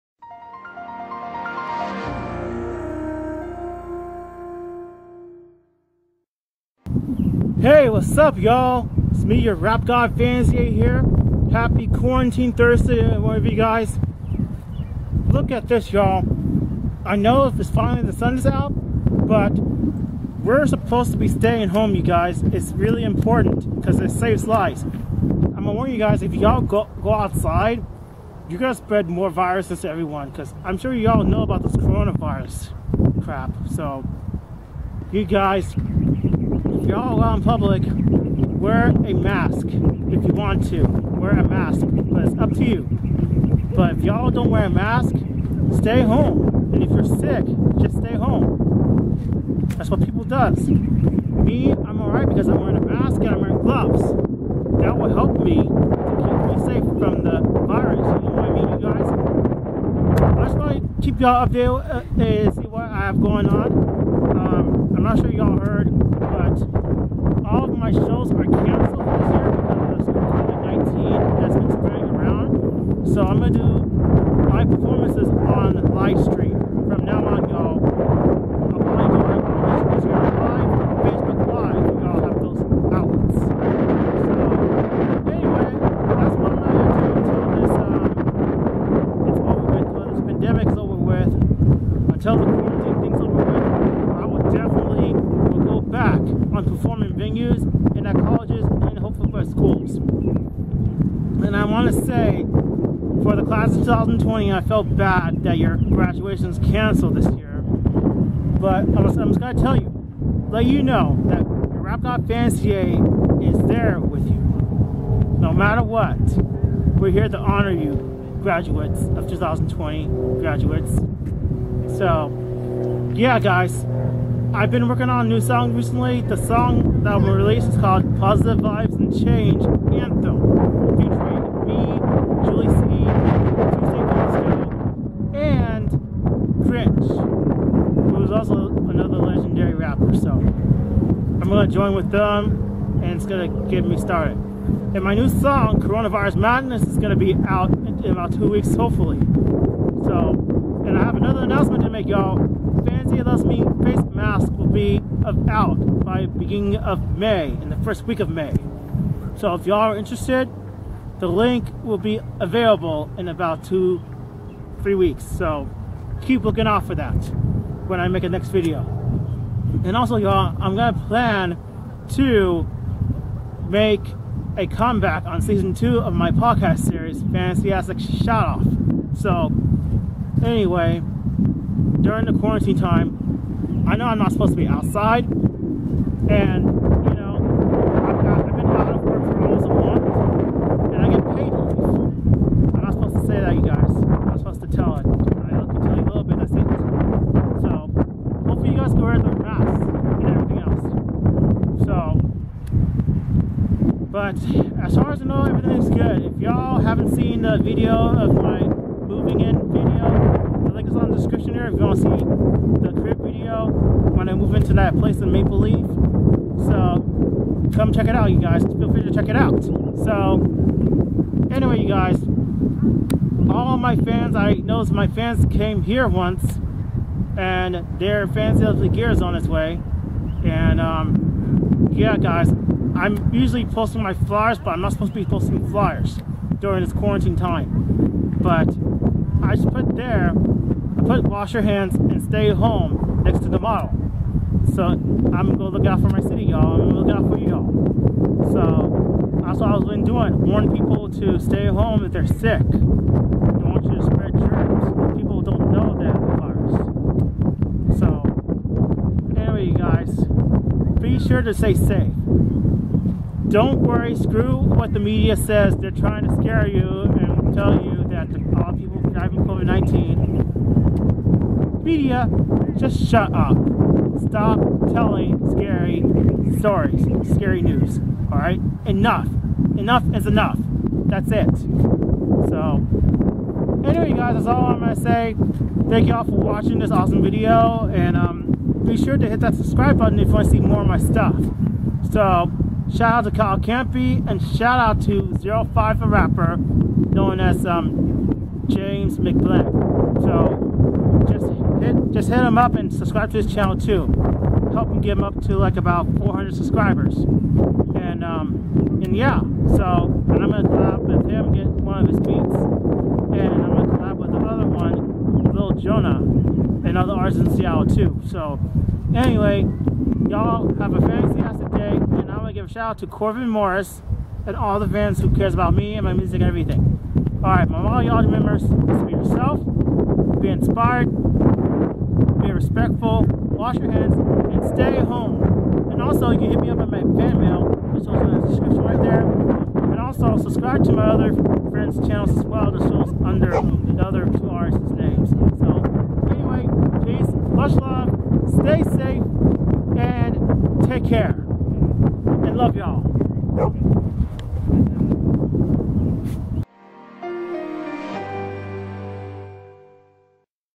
Quarantine Update 1
Here's the quarantine update video to tell y'all what i've been doing during the lockdown order by Governor Inslee and what I keep reminding people to stay home if they're still sick. Enjoy watching!!